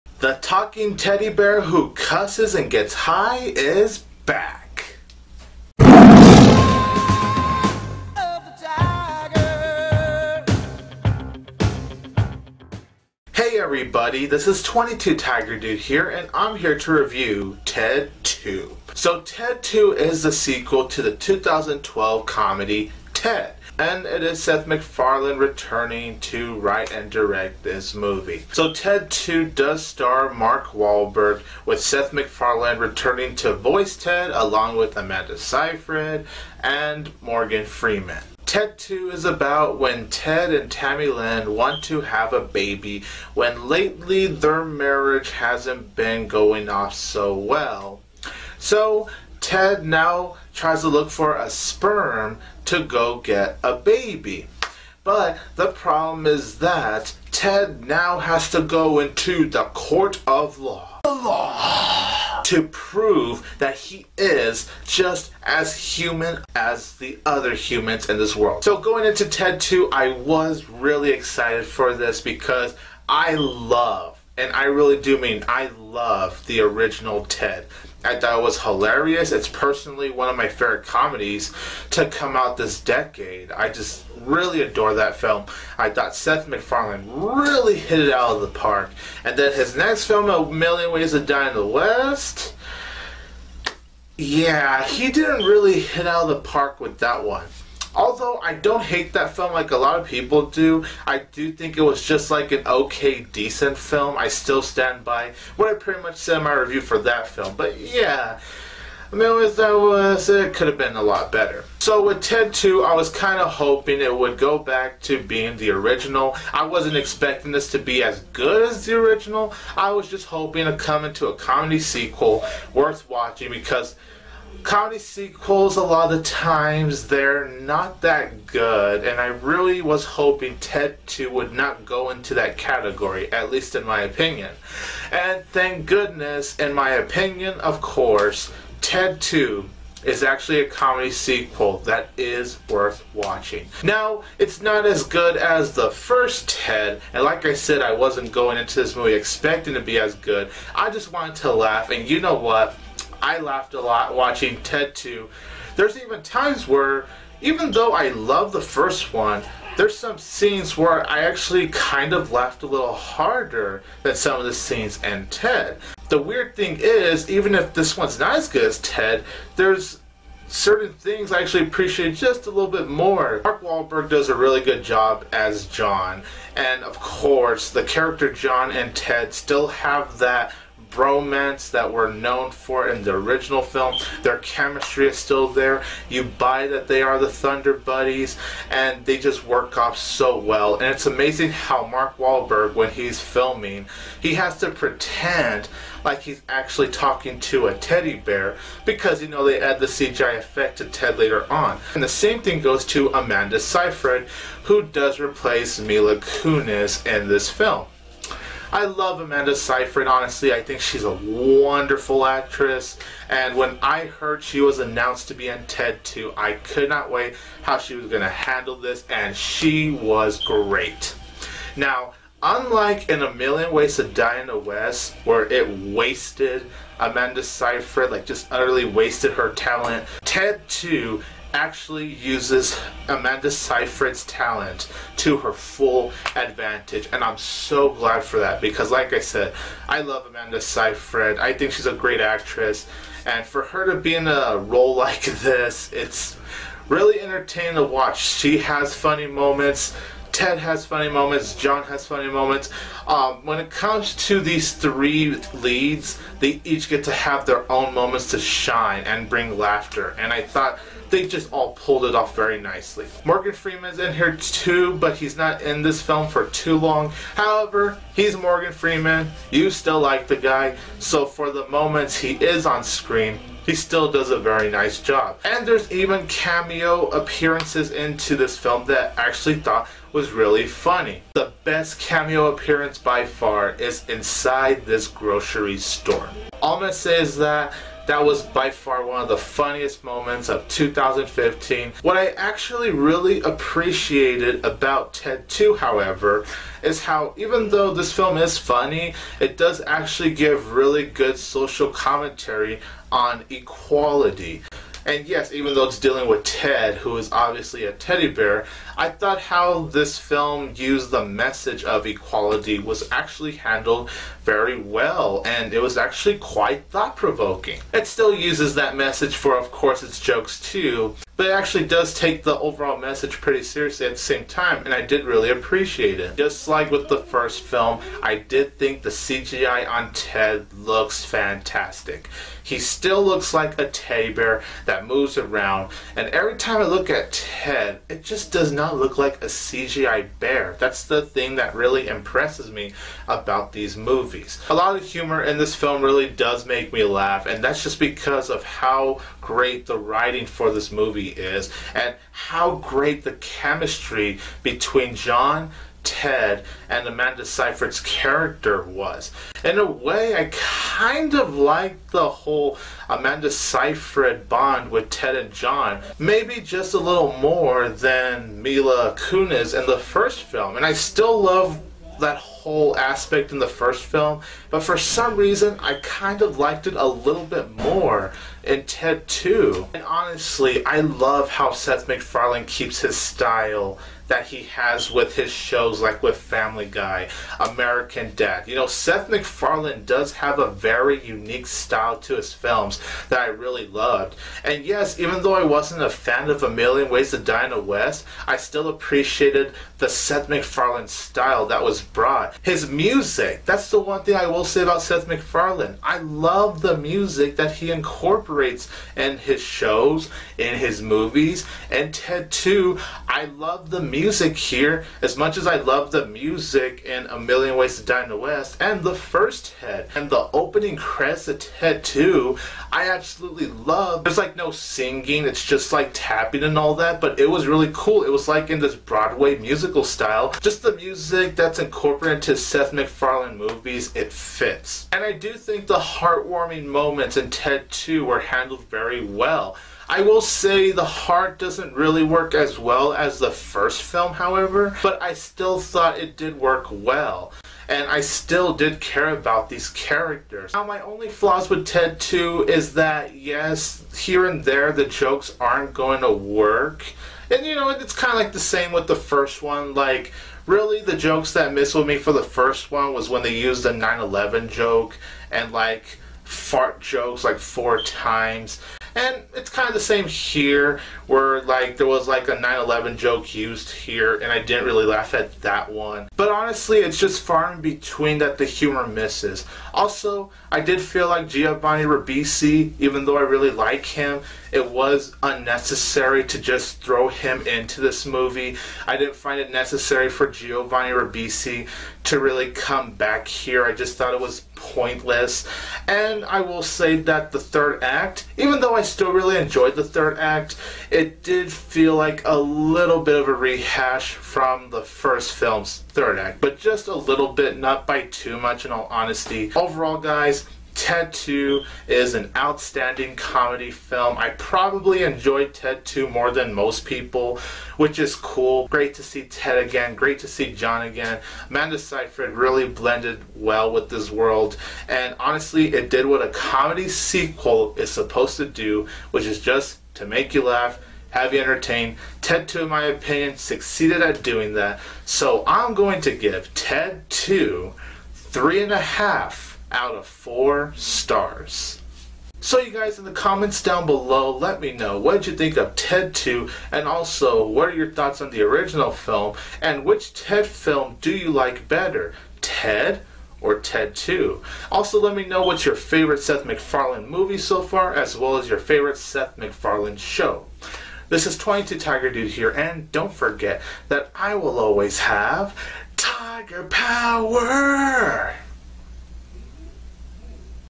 Ted 2 movie review
22tigerdude reviews the comedy sequel to the 2012 R-rated comedy "Ted". Starring Seth MacFarlane (voice of Ted), Mark Wahlberg, Amanda Seyfried, Giovanni Ribisi, and Morgan Freeman. What are his thoughts on this film? find out here.